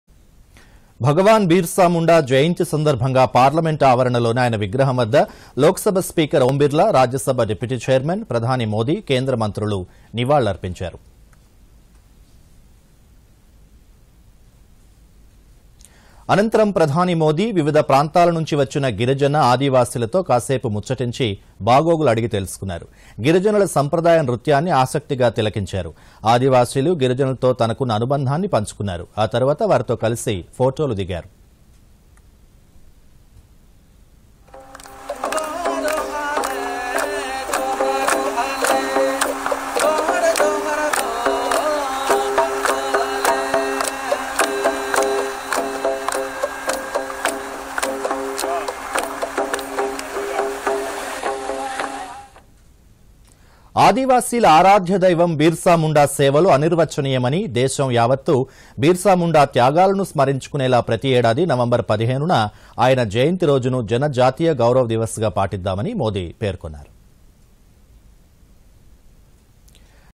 PM Modi pays tributes to Bhagwaan Birsa Munda Ji in Parliament | 🟥 DD News Telangana
భగవాన్ బిర్సా ముండా జయంతి సందర్భంగా పార్లమెంట్ ఆవరణలోని ఆయన విగ్రహం వద్ద లోక్ సభ స్పీకర్ ఓం బిర్లా, రాజ్యసభ డిప్యూటీ చైర్మన్, ప్రధాని మోదీ, కేంద్ర మంత్రులు నివాళులర్పించారు.

అనంతరం ప్రధాని మోదీ..  వివిధ ప్రాంతాల నుంచి వచ్చిన గిరిజన, ఆదివాసీలతో కాసేపు ముచ్చటించి బాగోగులు తెలుసుకున్నారు. గిరిజనుల సంప్రదాయ నృత్యాన్ని ఆకస్తిగా తిలకించారు. ఆదివాసీలు, గిరిజనులతో తనకున్న అనుబంధాన్ని పంచుకున్నారు. ఆ తర్వాత వారితో కలిసి ఫోటోలు దిగారు.

ఆదివాసీల ఆరాధ్య దైవం బిర్సా ముండా సేవలు అనిర్వచనీయమని.. దేశం యావత్తు బిర్సా ముండా త్యాగాలను స్మరించుకునేలా ప్రతీ ఏడాది నవంబర్ 15న ఆయన జయంతి రోజును    జన జాతీయ గౌరవ దివస్ గా పాటిద్దామని మోదీ పేర్కొన్నారు.